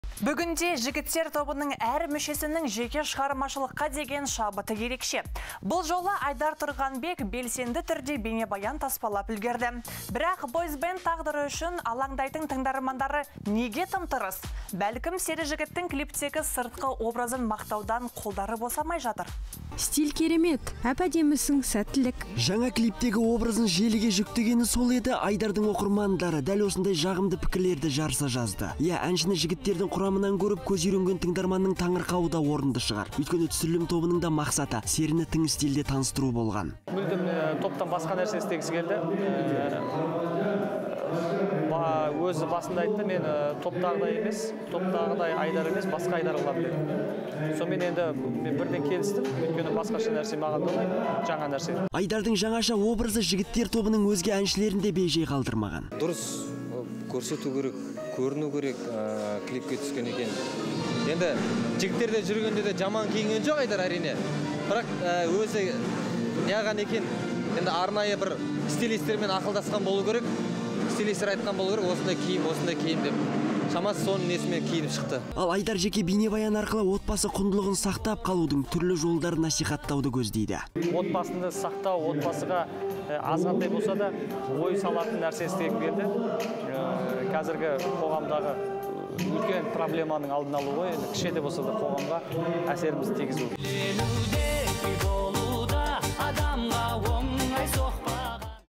Жеке клипінде ерекше стилімен тәнті қылған Айдар Тұрғанбек «Жігіттердегі» жұмысын тоқтатты ма?!
Бүгінде «Жігіттер» тобының әр мүшесінің жеке шығармашылыққа деген шабыты ерекше. Бұл жолы, Айдар Тұрғанбек белсенді түрде бейнебаян таспалап үлгерді. Бірақ, бойзбенд тағдыры үшін алаңдайтын тыңдармандары неге тым-тырыс? Бәлкім, сері жігіттің клиптегі сыртқы образын мақтаудан қолдары босамай жатыр.